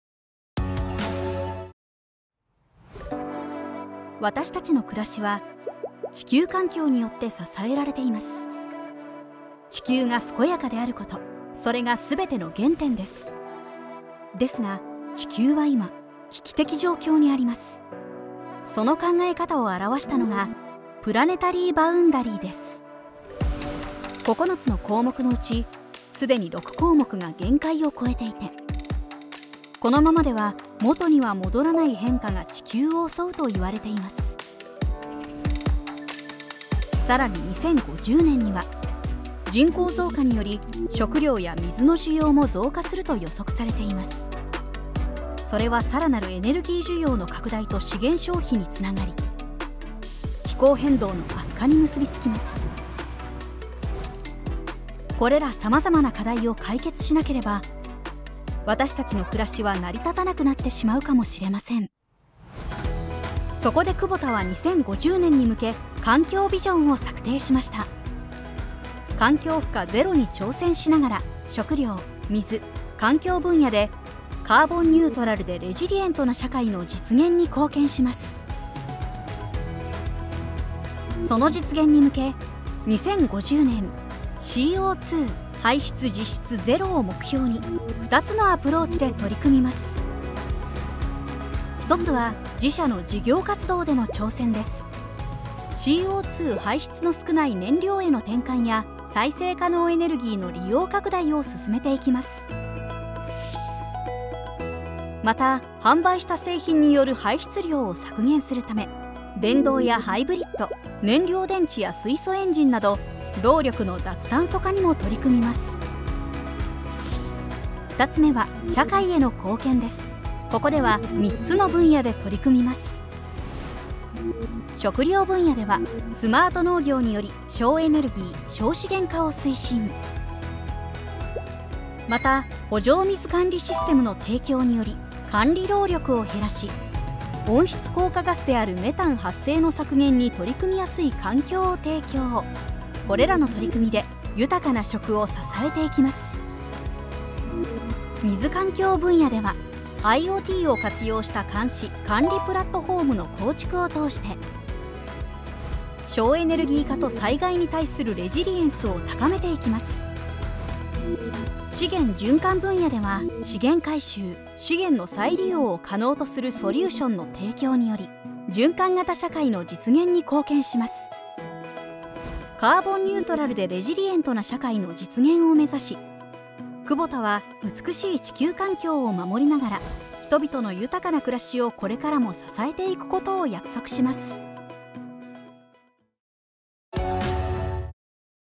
環境ビジョン
クボタグループが掲げる『環境ビジョン』のご紹介です。現在、食料問題や地球温暖化などの地球規模の社会課題により、将来の不確実性が増しています。そこで2021年にクボタグループは「2050年に向けて環境面からのありたい姿」を表す『環境ビジョン』を掲げました。クボタグループは、環境負荷ゼロに挑戦しながら、「食料・水・環境」分野でカーボンニュートラルでレジリエントな社会の実現に貢献します。